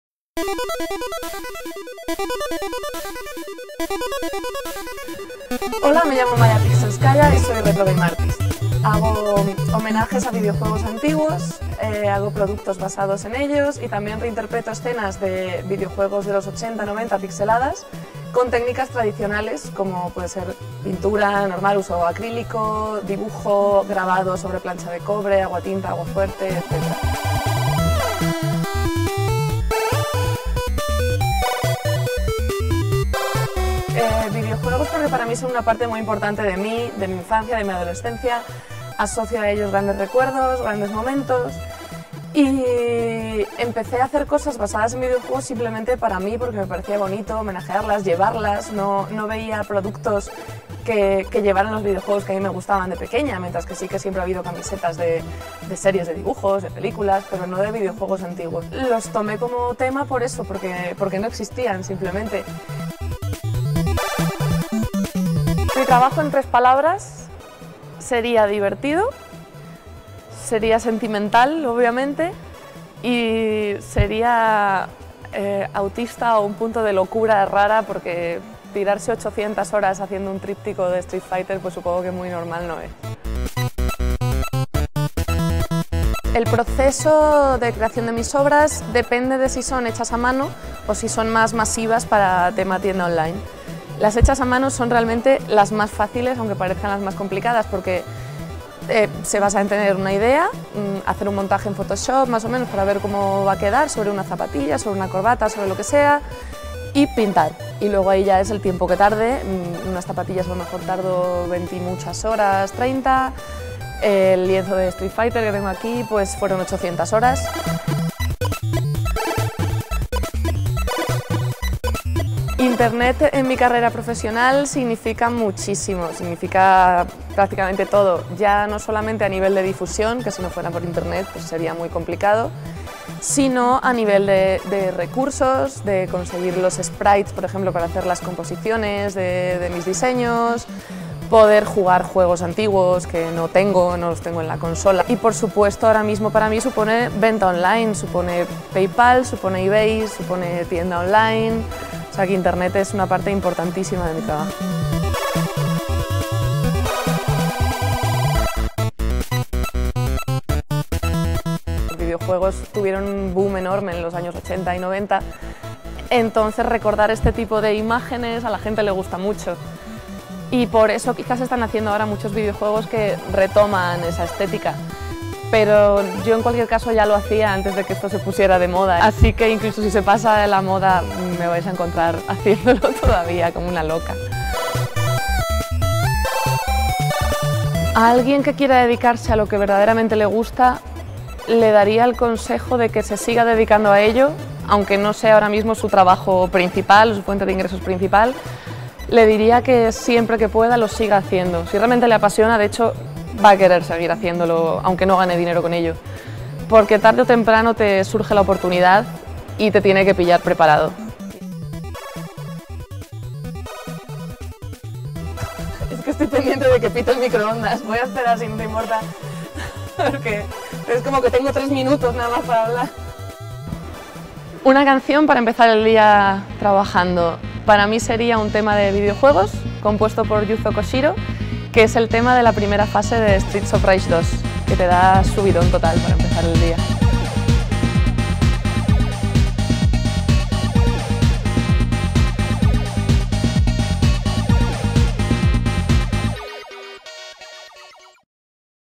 TGB Local Heroes- Maya Pixelskaya
LocalHeroes is back!! Y esta vez con una artista con la que es imposible no quedarse boquiabierto.Creaciones inspiradas en la estética de los videojuegos retro que tanto nos han flipado de pequeños.
Maya Pixelskaya - RetroGame Art​  ¡Pasen y vean!  ‪